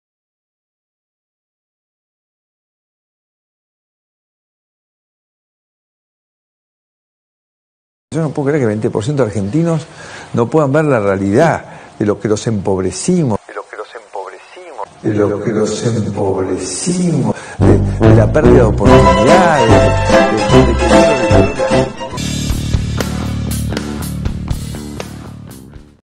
Mauricio Macri con Majul indignado
Cuando un político al que sus seguidores, lo definen como un estadista, creen en su palabra porque destacan que él fue el único que decía la verdad aunque duela, se encuentra cómodo con un amigo periodista como Luis Majul que no solo militó sus ideas, sino que es parte interesada del macrismo, donde trabajan su esposa y otros familiares, indignado hace estas afirmaciones, hay que recordarlas.

Dice el refrán «confessio per relevium de testimonio«, no hay mucho por agregar, gracias Mauricio por indignarte con los que no entendieron tu gobierno y tus logros.